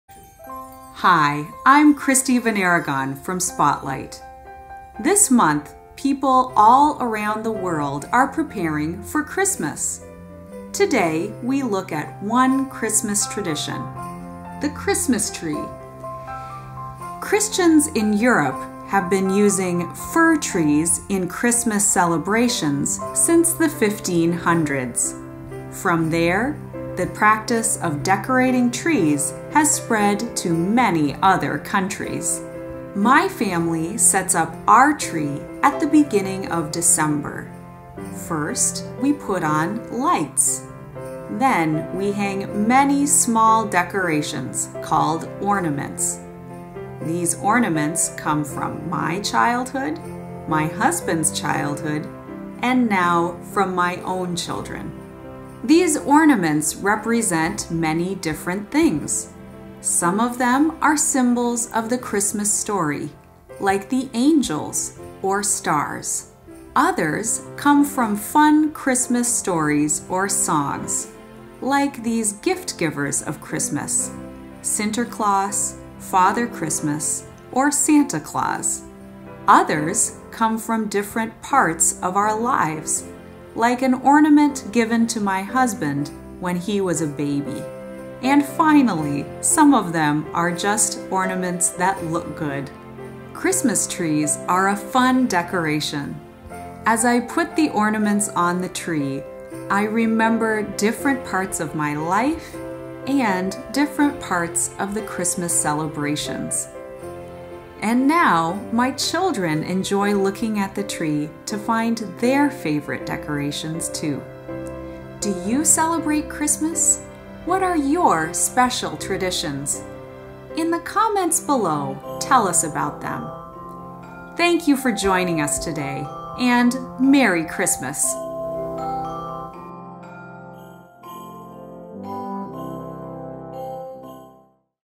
Decorating a Christmas Tree | practice English with Spotlight
Many Christians have begun thinking about the holiday of Christmas! Some have begun setting up decorations like Christmas Trees. Join Christy as she tells about decorating a Christmas Tree.